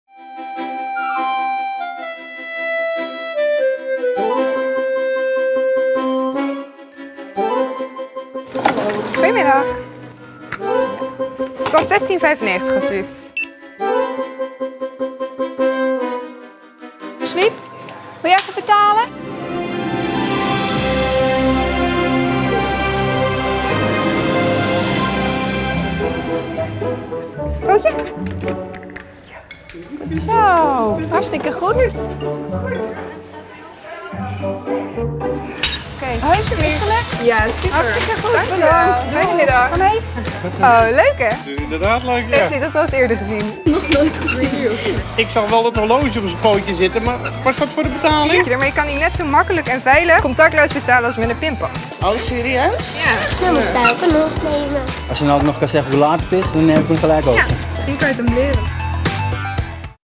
Wearables: hond Swiep betaalt met zijn horloge
Betalen met een wearable is zo makkelijk, dat je zelfs je hond voor een boodschap kunt sturen!